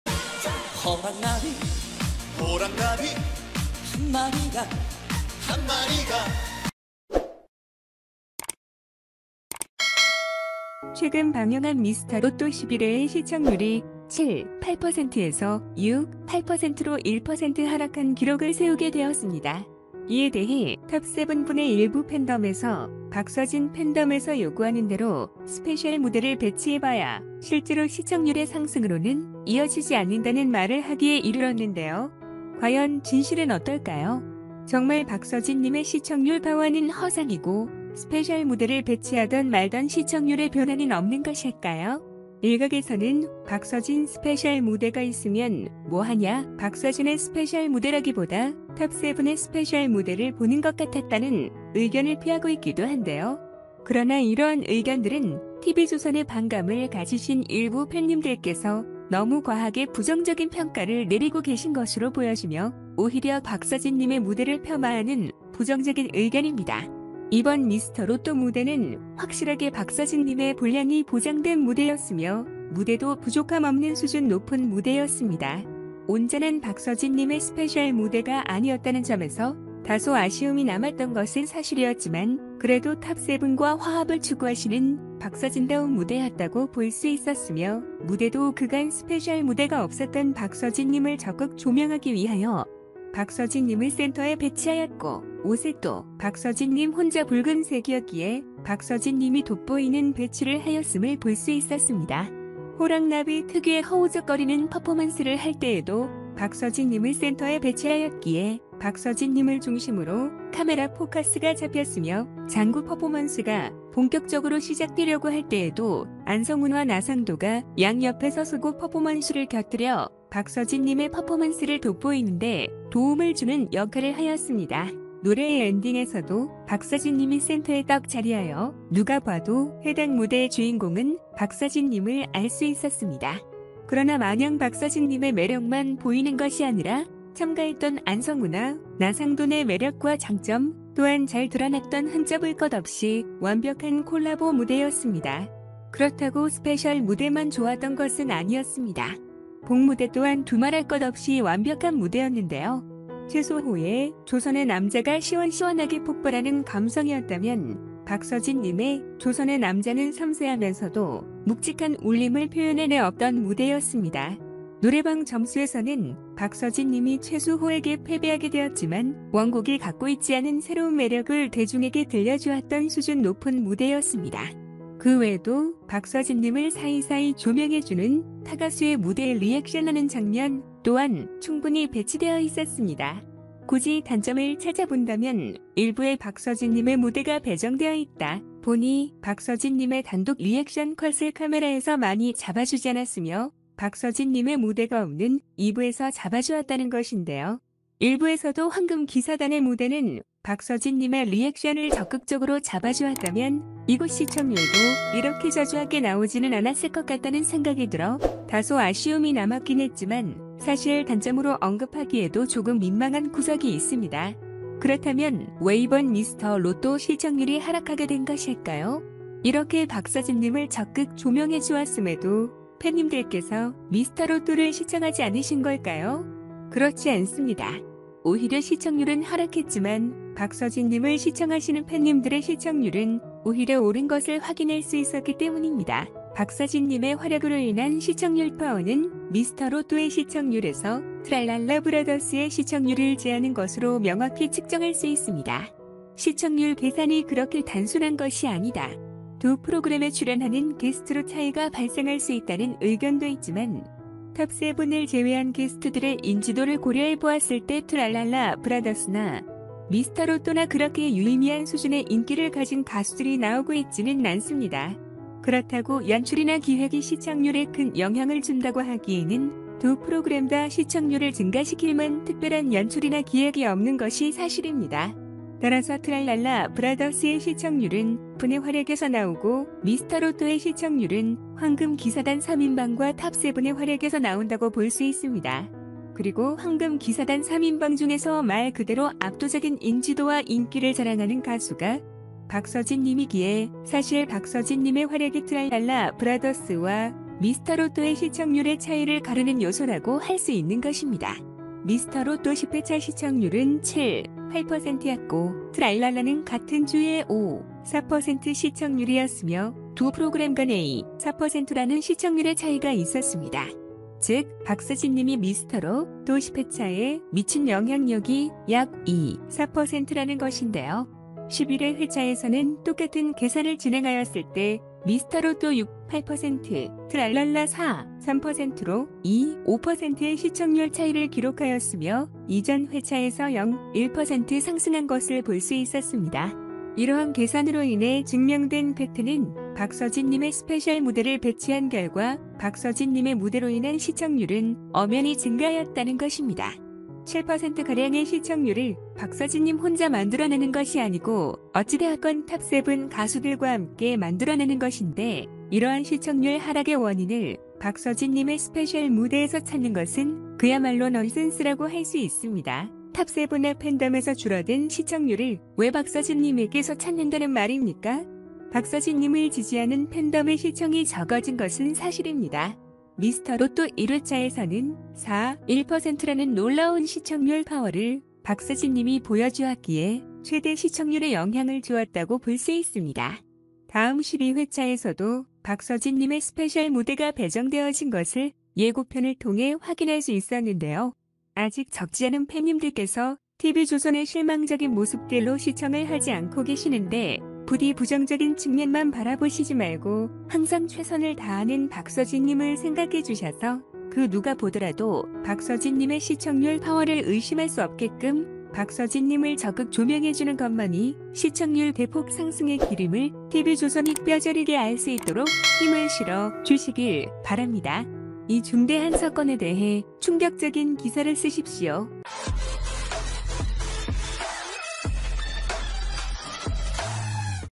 [특별 뉴스] 박서진 님의 무대가 미스터 로또 시청률에 미치는 영향☝️팬덤 TOP7은 왜 박서진 때문에 시청률이 떨어졌다고 생각하는 걸까? 진짜 원인이 밝혀진다!
채널 KrVlog 247에 오신 것을 환영합니다.